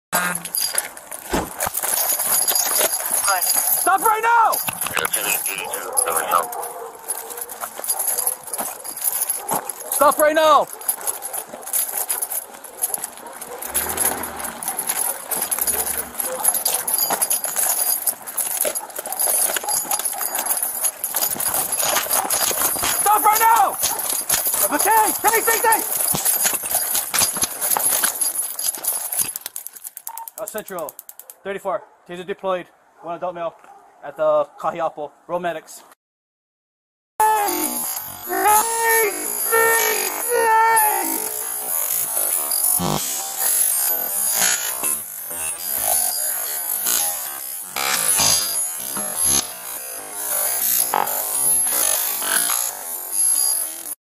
GRAPHIC: Bodycam footage released by MPD shows moment officer tases apparent car theft suspect
A grieving Maui family is demanding answers after their loved one died on Thursday, one week after being tased by police.